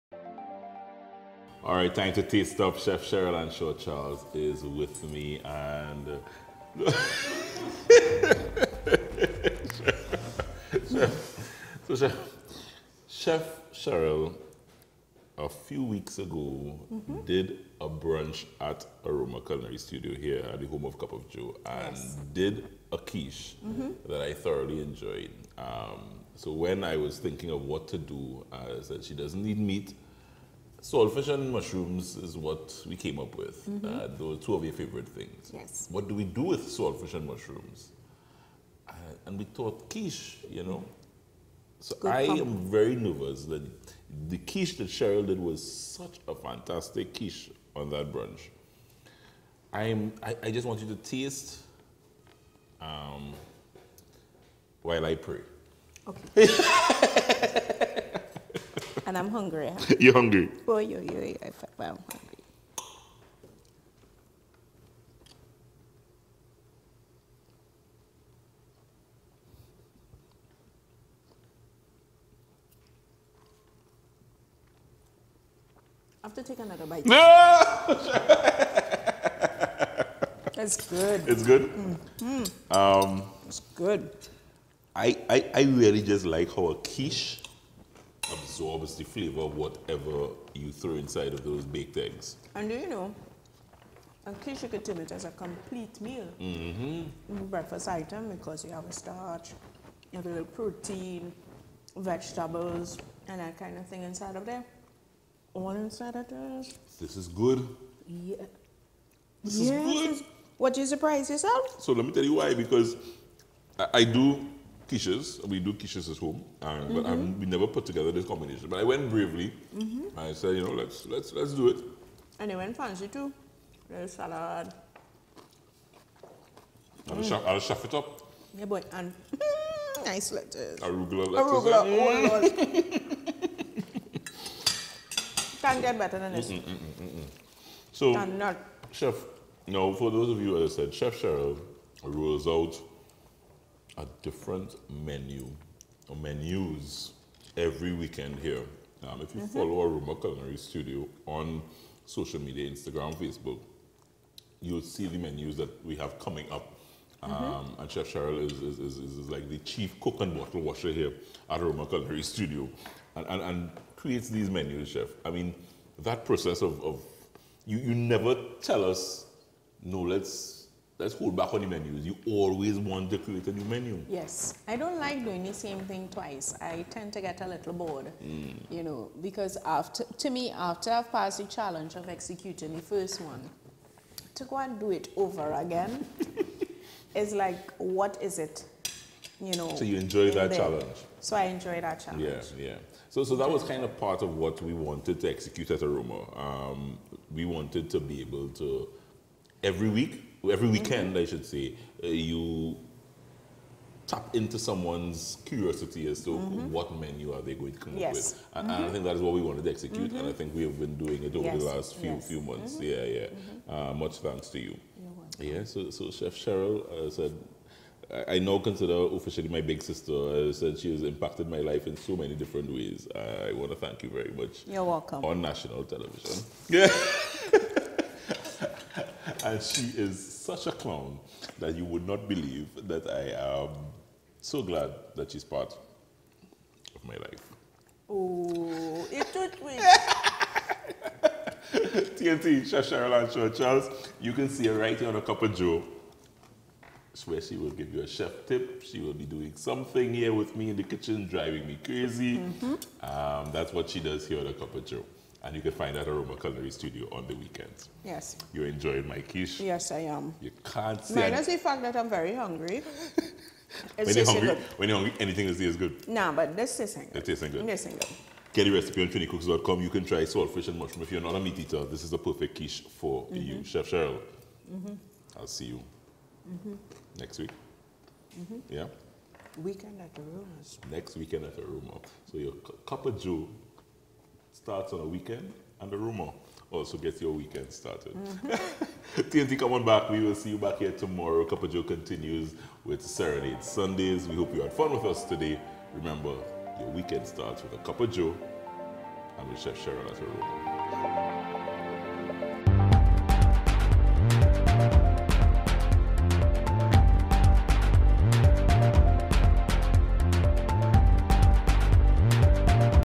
Chef Cheryl Ann Shortt-Charles at theTasting Table
After flipping roles in the Cup of joe kitchen, Chef Cheryl - Ann Shorrt Charles tastes the Saltfish and Mushroom Quiche that Joel made for her.